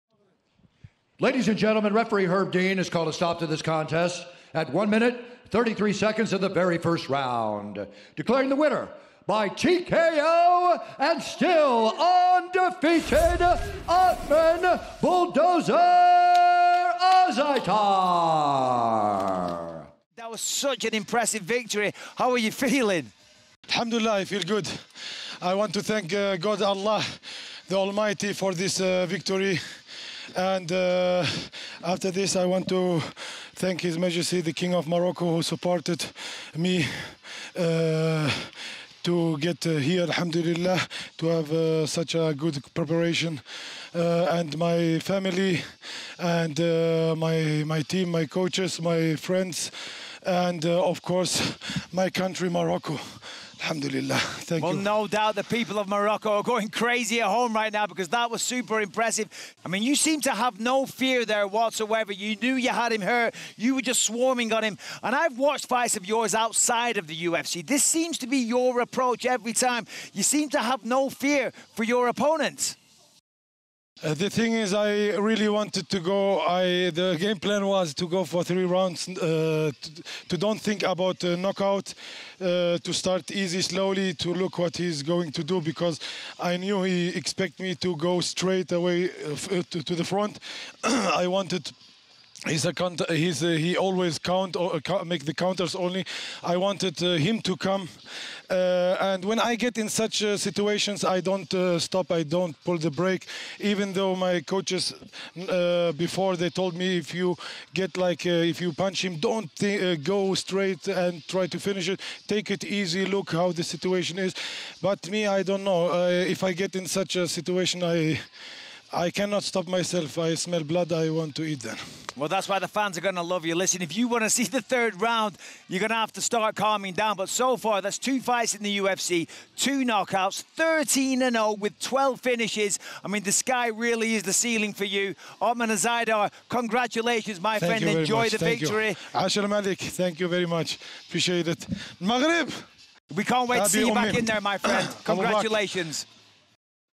UFC Vegas 10: Ottman Azaitar Interview after 1:33 KO
Ottman Azaitar stops his opponent early in the first round to remain undefeated at UFC Vegas 10.